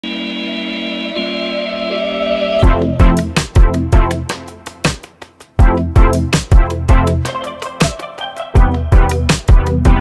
6 Ways To Make Passive Income As A Photographer
1. Sell prints
2. Offer associate photographers
3. Sell presets
4. Sell guide templates for other photographers
5. Get commission for an Amazon storefront

Follow for more photography tips! for more wedding photography content!